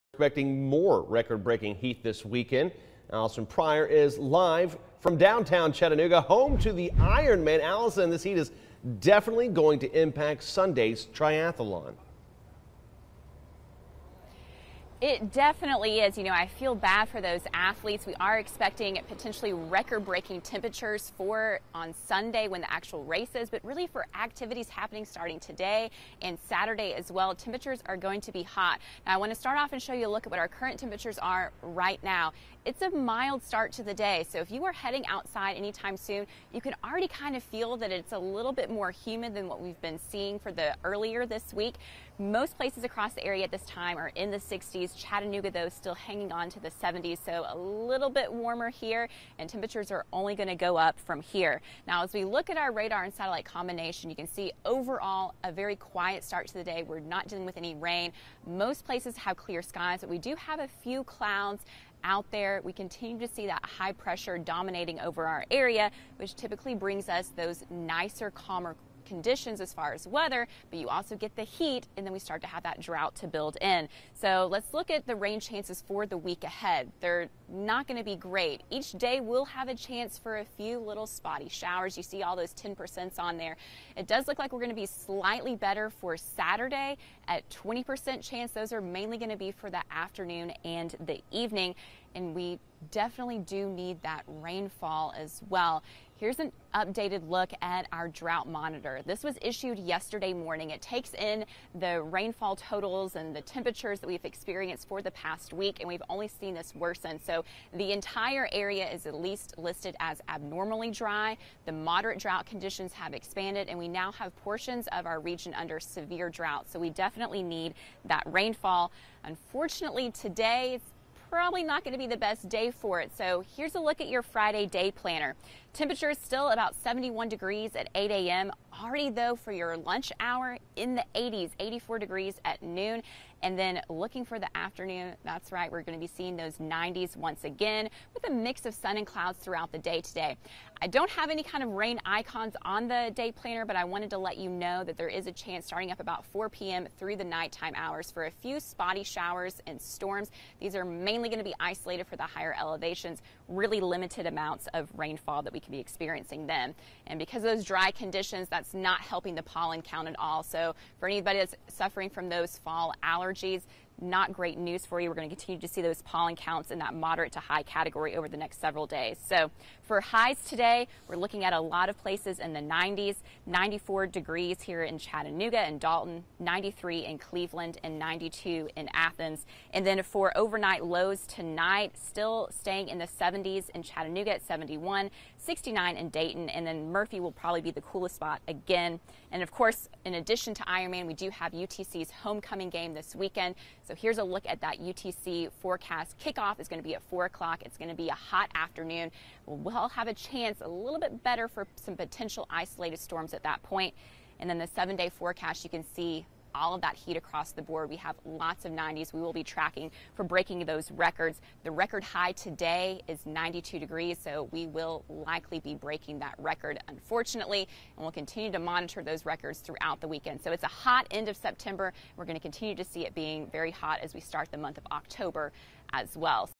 Alison Pryor's Friday morning forecast
ᐸpᐳTemperatures will start off mild this morning. Watch for patchy foggy reducing visibilityᐸ/pᐳ